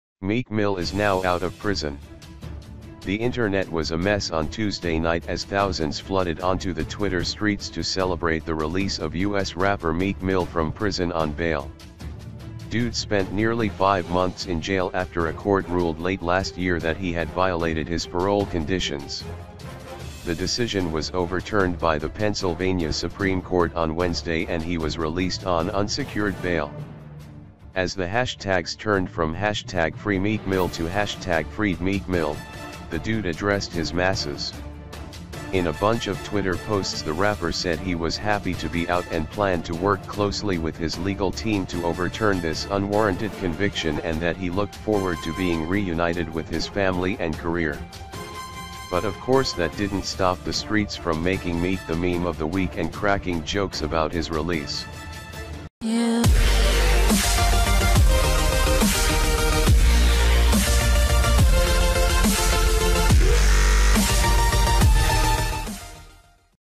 Meek Mill is now out of prison
The internet was a mess on Tuesday night as thousands flooded onto the Twitter streets to celebrate the release of US rapper Meek Mill from prison on bail.
Dude spent nearly five months in jail after a court ruled late last year that he had violated his parole conditions. The decision was overturned by the Pennsylvania Supreme Court on Wednesday and he was released on "unsecured bail."
As the hashtags turned from FreeMeekMill to FreedMeekMill, the dude addressed his masses. In a bunch of Twitter posts the rapper said he was happy to be out and planned to work closely with his legal team to overturn this "unwarranted conviction" and that he looked forward to being reunited with his family and career.
But of course that didn't stop the streets from making Meek the meme of the week and cracking jokes about his release.